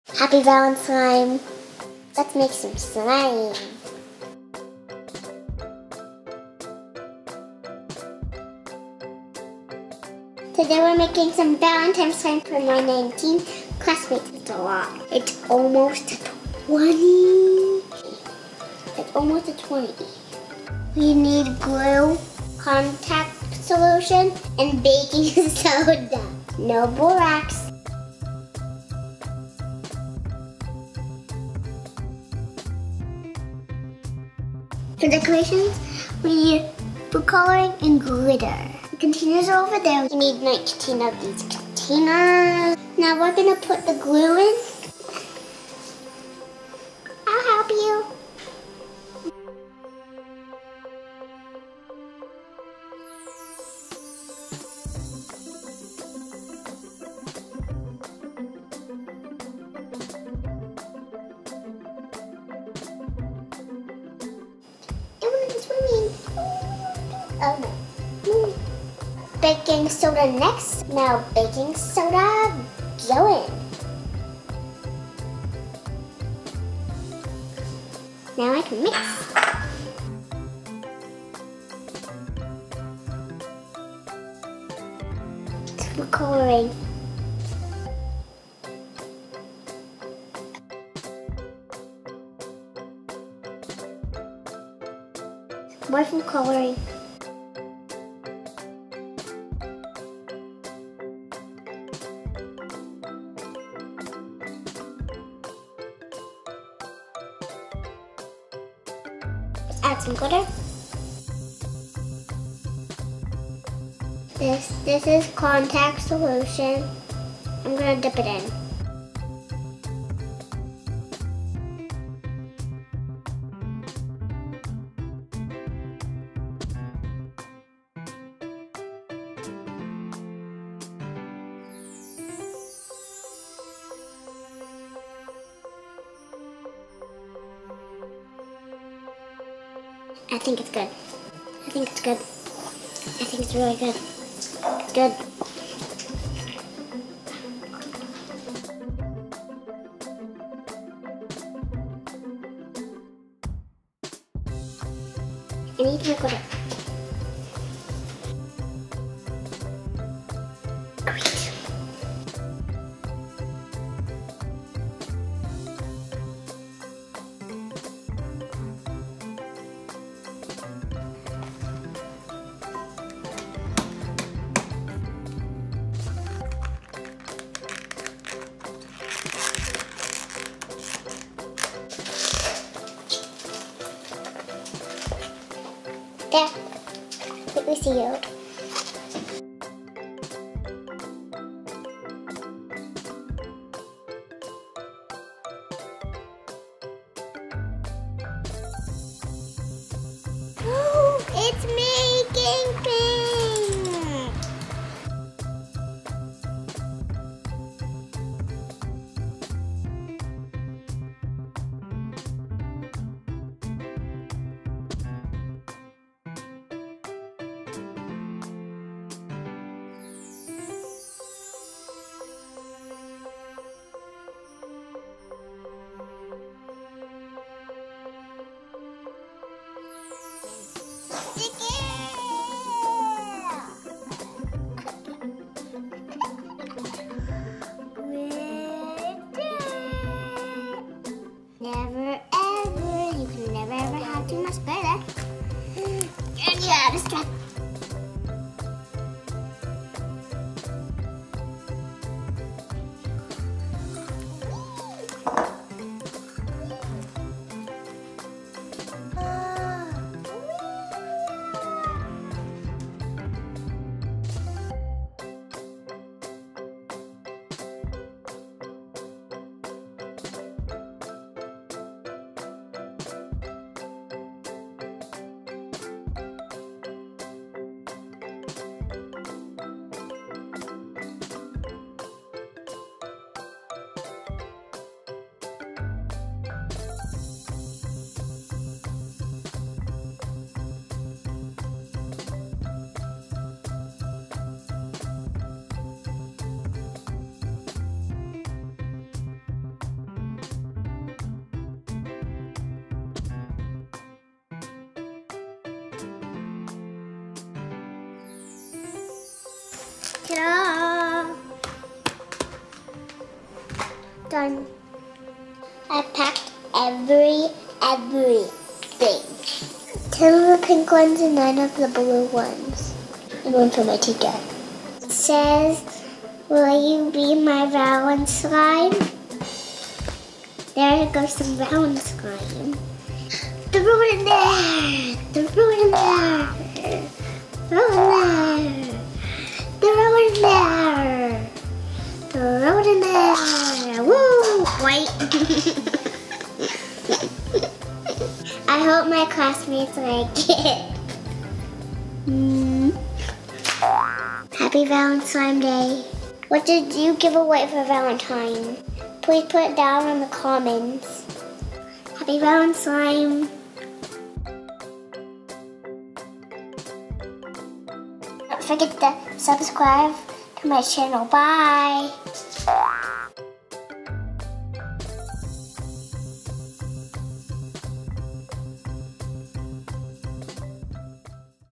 Easy and Satisfying Slime without Borax Tutorial | Saline Solution and Baking Soda
I had a lot of fun making Valentine's giveaway slime for my classmates. This easy and satisfying slime tutorial has no borax in it and uses a popular recipe with baking soda and contact solution. Happy Valenslime!!!

Each slime "serving" or container in this video used one 4-oz bottle of Elmer's glue, 1 tsp. baking soda and 2-3 tbsp of saline solution, divided (contact lens solution... make sure it contains both sodium borate and boric acid as ingredients).

We scaled this recipe up to ~20 for all of Cathleen's classmates plus teacher. They loved it!

For the chemistry behind slime, please head to the American Chemical Society website for a fantastic and legit explanation